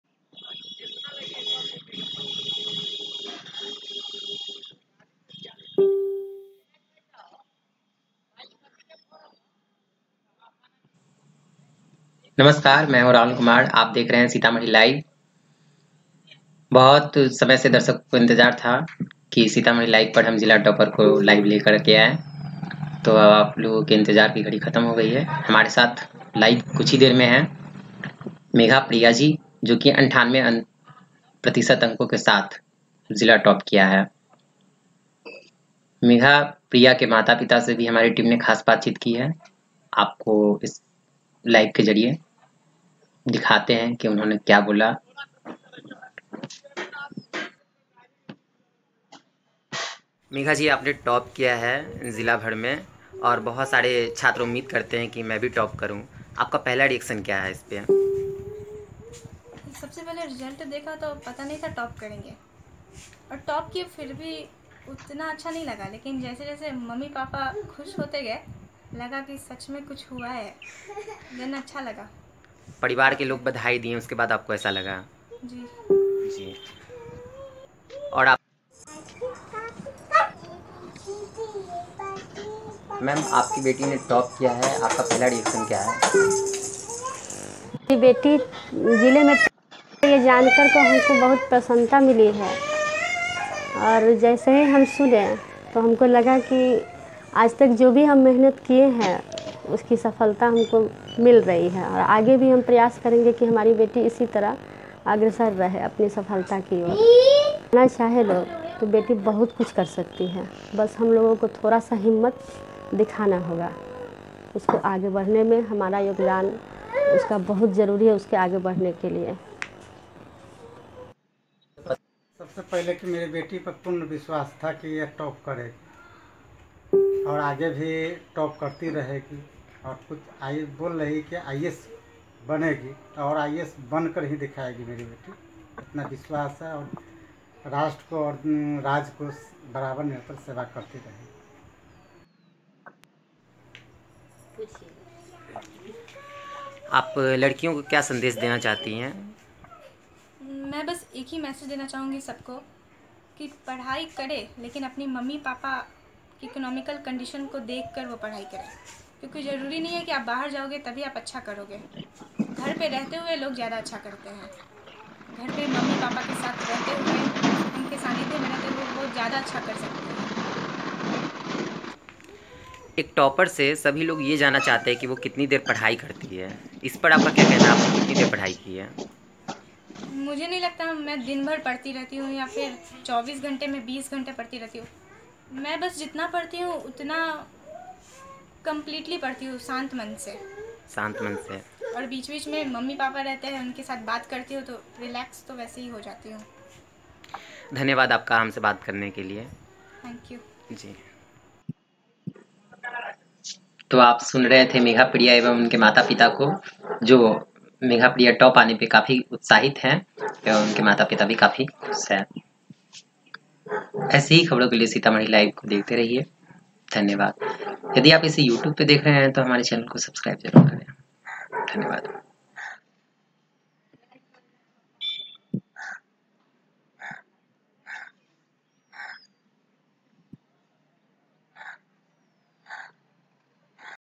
CBSE 10th Sitamarhi Topper Megha Priya with Exclusive Interview
IAS बनना चाहती हैं मेघा प्रिया 😊
मंगलवार को CBSE 10वीं के नतीजे घोषित होने के बाद छात्रों के बीच काफी उत्साह है. सीतामढ़ी जिला में 98% अंक के साथ मेघा प्रिया ने जिला में प्रथम स्थान प्राप्त किया है. मेघा प्रिया शहर की सरस्वती विद्या मंदिर की छात्रा है और कोचिंग चकमहिला चौक स्थित स्कॉलर हब में किया है. मेघा दो बहन और एक भाई में सबसे बड़ी है. नेहा ने बताया कि वह आगे पढ़ लिखकर आईएएस बनकर देश की सेवा करना चाहती है. मेघा की सफलता से इनके माता-पिता काफी खुश है साथ में इनके पैतृक गांव में भी खुशी की लहर है.